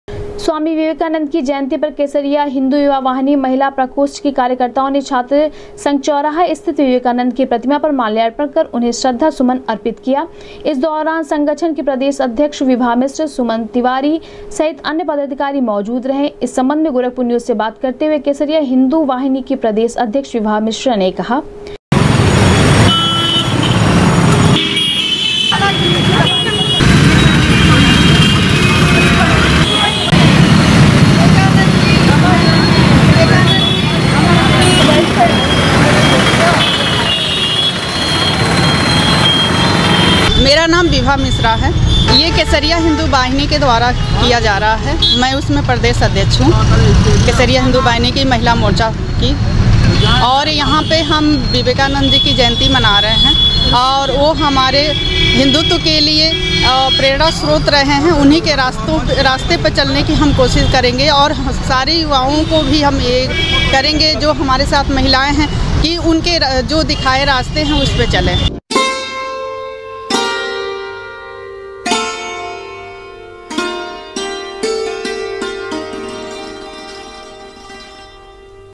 स्वामी विवेकानंद की जयन्ती पर केसरिया हिन्दू युवा वाहिनी महिला प्रकोष्ठ की कार्यकर्ताओ ने छात्र संघ
स्वामी विवेकानंद की जयन्ती पर केसरिया हिन्दू युवा वाहिनी महिला प्रकोष्ठ की कार्यकर्ताओ ने छात्र संघ चौराहा स्थित विवेकानद की प्रतिमा पर किया माल्यार्पण,दी श्रद्धाजंलि।।